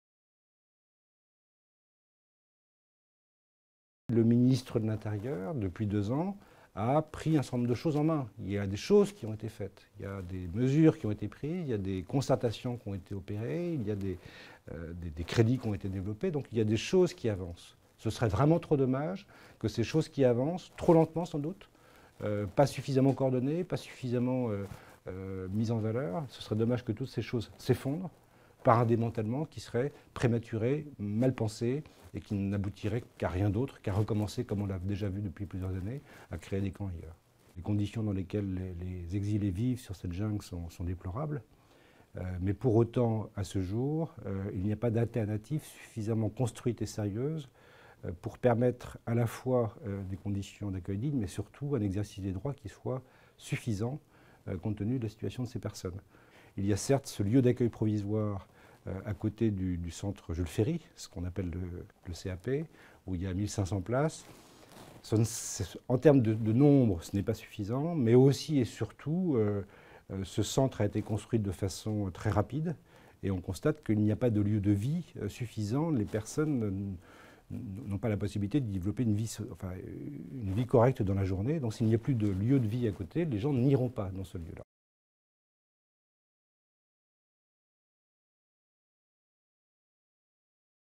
Démantèlement de la jungle de Calais: les associations inquiètes
Une semaine après la décision de l'Etat de faire partir les migrants d'une grande partie de la "Jungle" de Calais, les associations affûtent leur réponse pour tenter d'empêcher l'opération, face à des pouvoirs publics qui jouent la carte de l'apaisement.